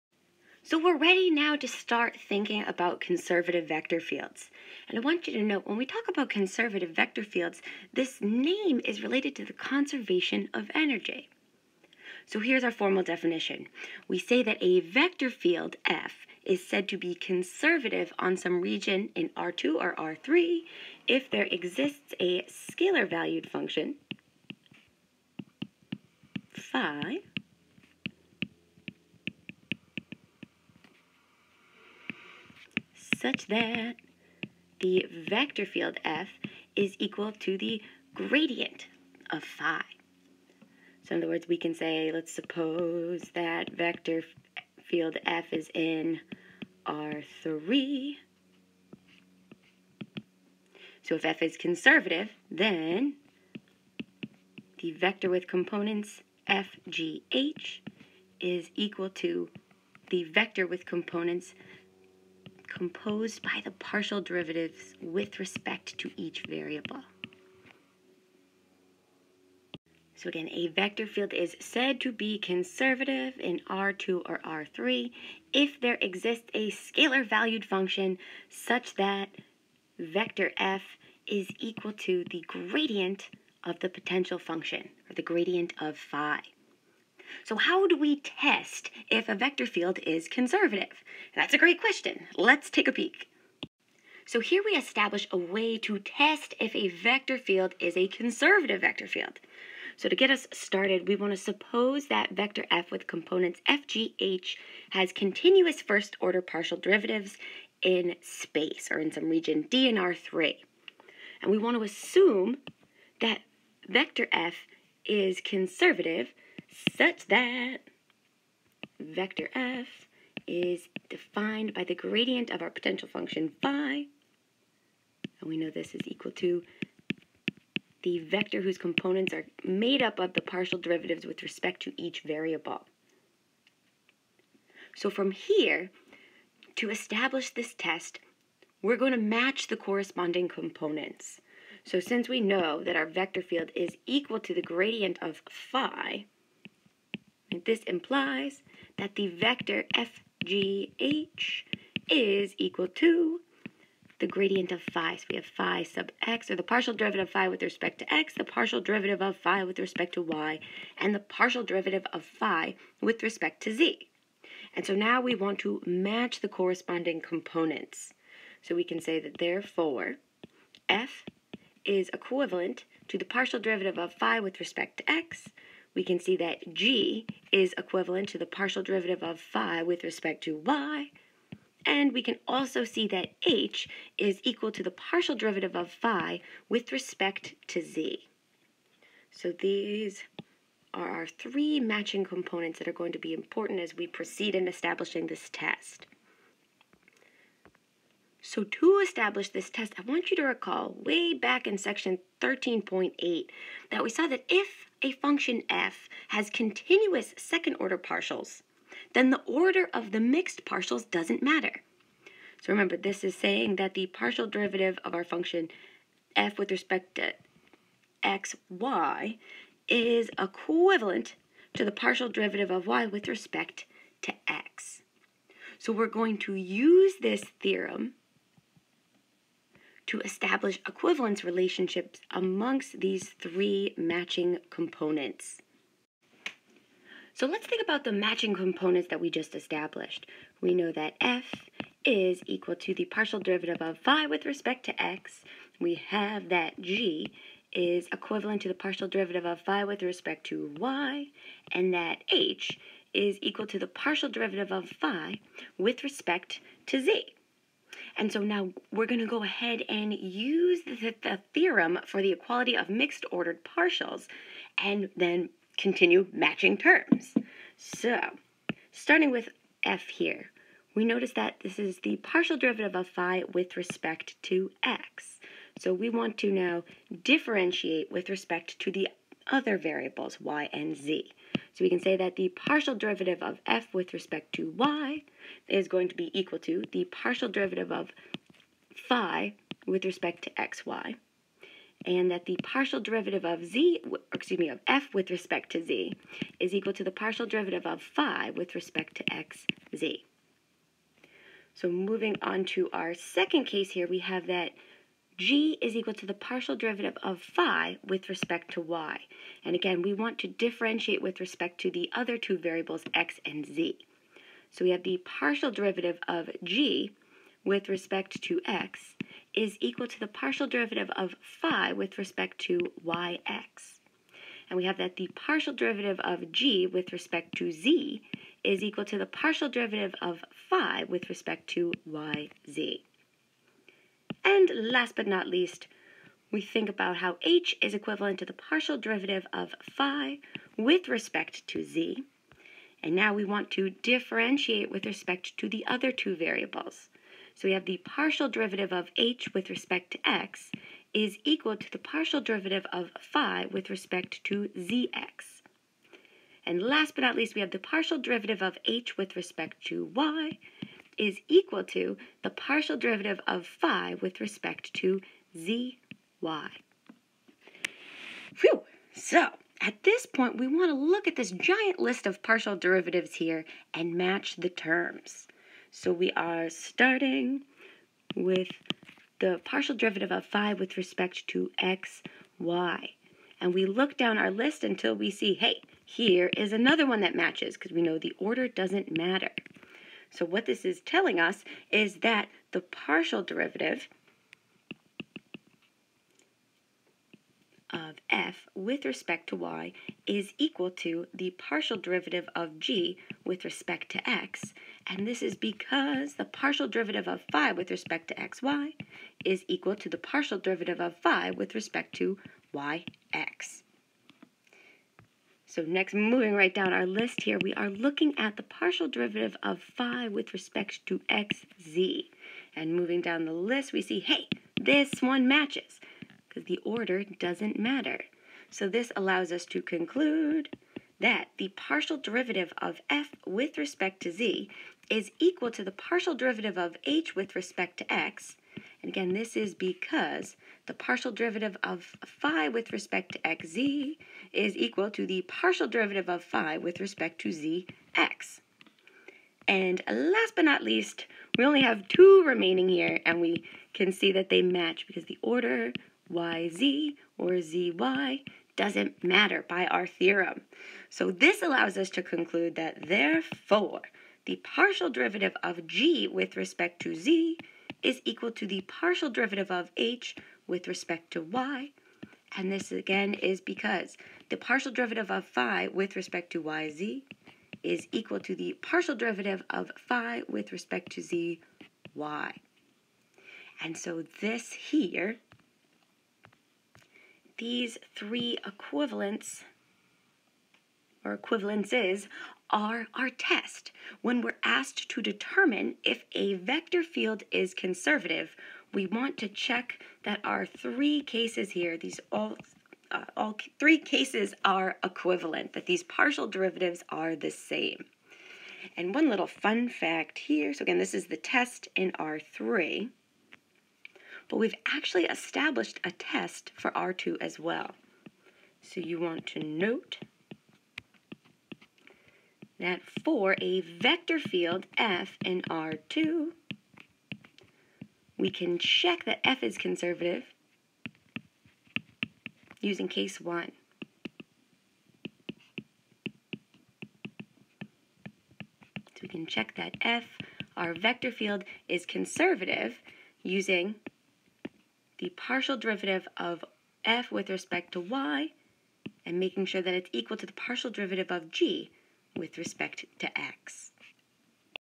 Test for Conservative Vector Fields
Definition of a Conservative Vector Field. Introduction to the Test for Conservative Vector Fields in Two Dimensions & Three Dimensions.